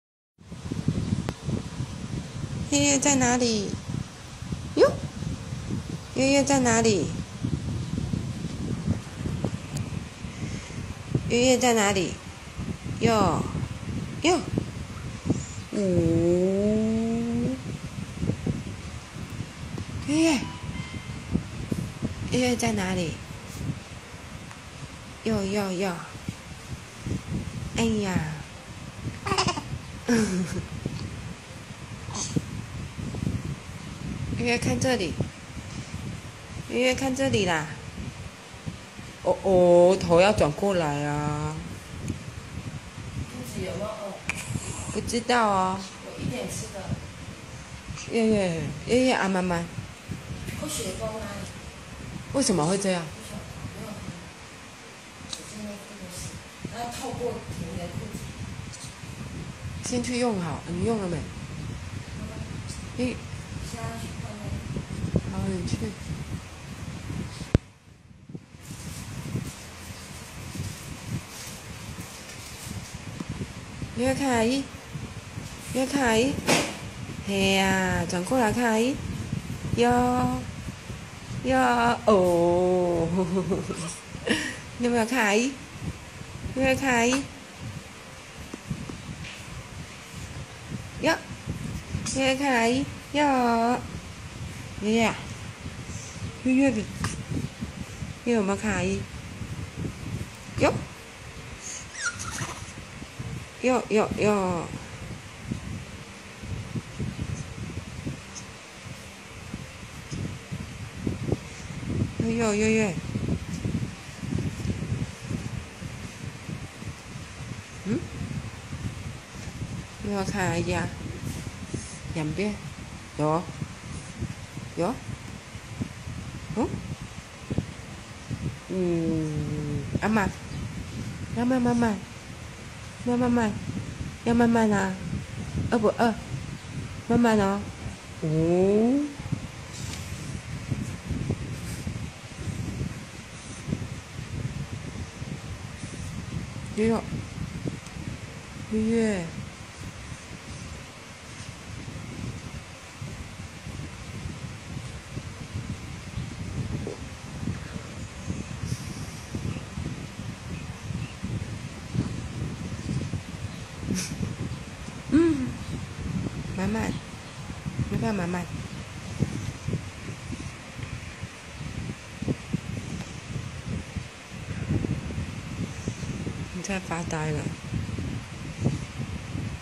岳岳在哪裡
阿姨在跟小岳岳聊天